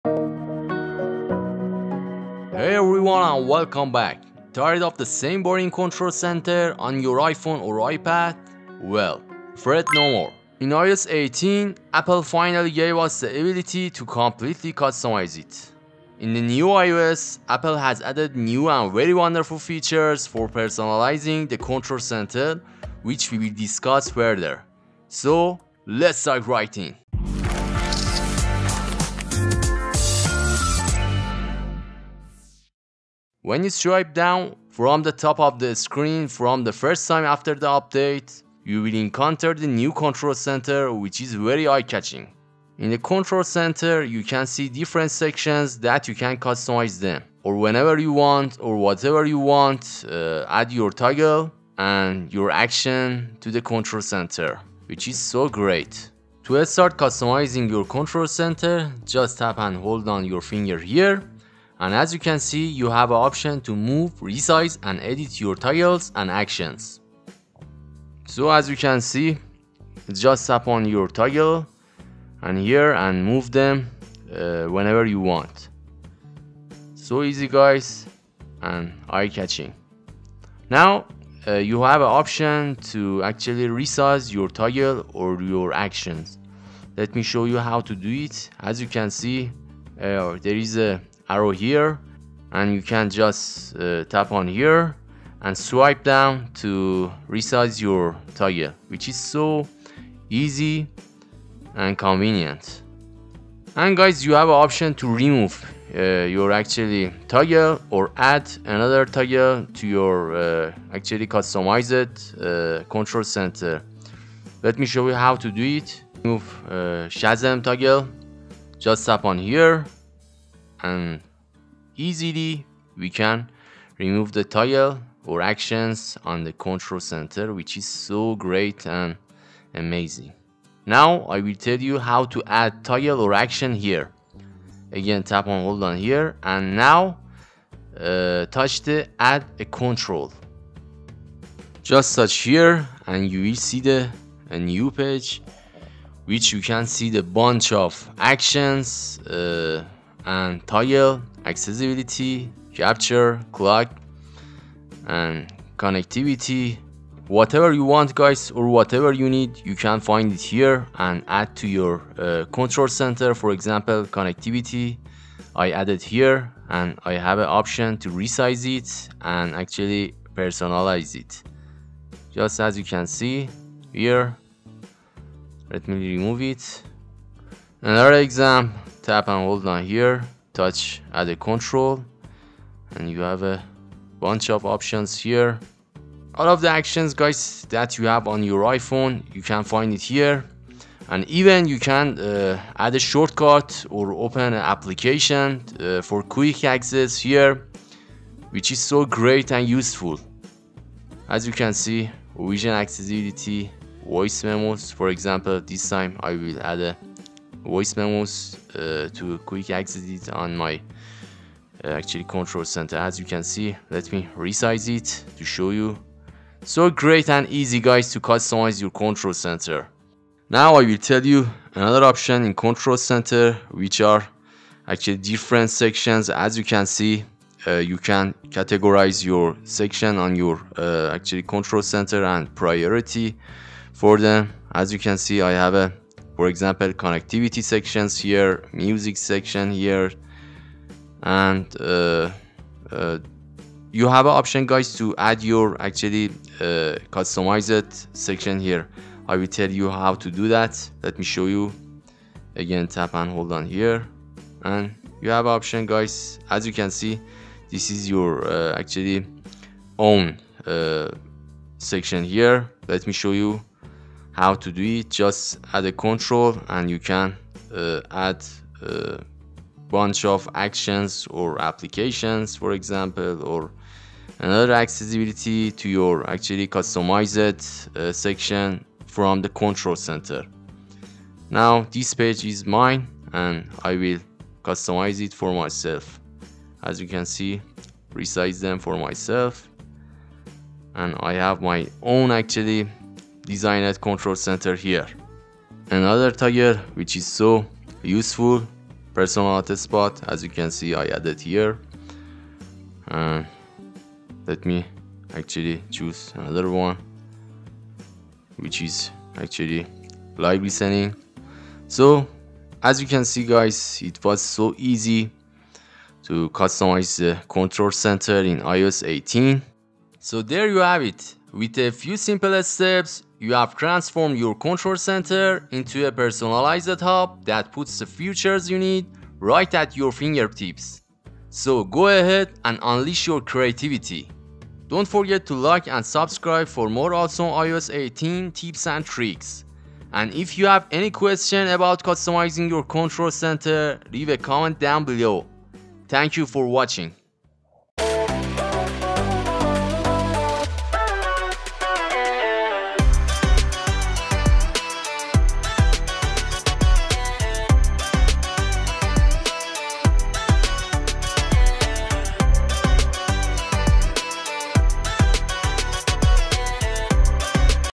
iOS 18 Control Center The ULTIMATE Customization Guide (NEW Update!)
In this video, I'll show you how to unleash the full potential of your iPhone with iOS 18's brand new Control Center!  Tired of the same old layout? This guide unlocks complete customization.  Learn how to add Music, Flashlight, Notes & MORE to your Control Center.  Plus, rearrange and resize everything for a setup that perfectly fits your needs. Watch now and transform your Control Center into a productivity powerhouse!

Frustrated with the default iPhone Control Center?  iOS 18 lets you customize it completely!  This video is your ULTIMATE guide: learn how to add Music, Flashlight, Notes & MORE!   Rearrange and resize for a perfect setup.  Watch now & unlock the full power of your Control Center!

Video Chapters
0:00 - Introduction to iOS 18 Control Center Customization
0:33 - Overview of the New Control Center Layout
1:00 - Resizing and Moving Control Center Tiles
2:12 - Adding and Removing Control Center Actions
3:43 - Creating Custom Sections in Control Center